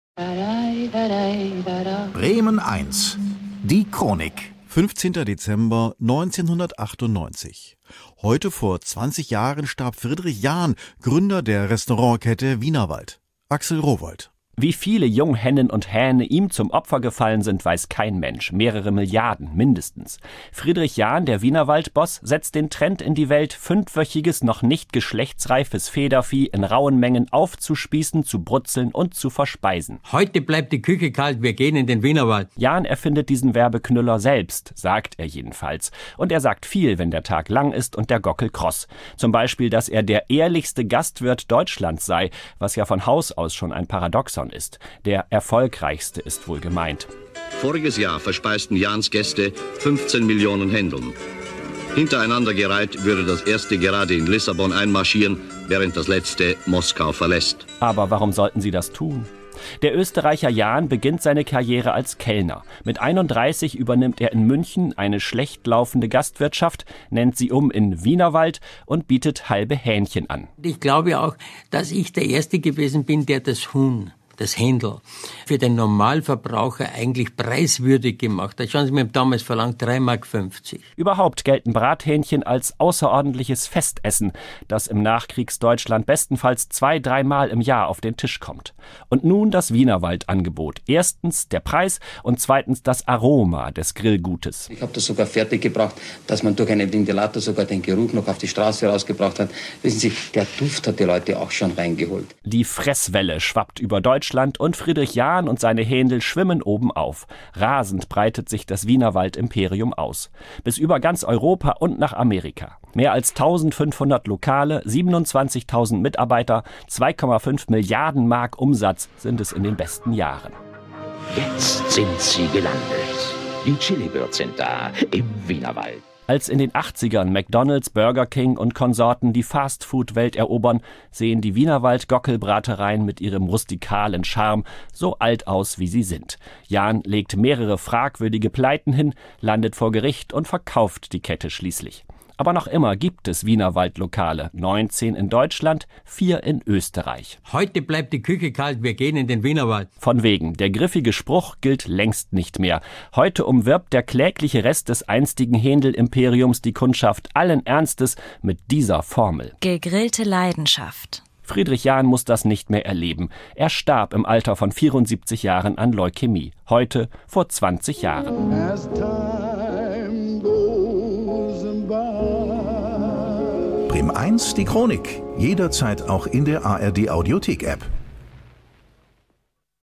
15.12.1998: Todestag Friedrich Jahn
Heute vor 20 Jahren starb der Gastronom Friedrich Jahn, Gründer der Kette „Wienerwald“.
Autor/-in:Axel Rowohlt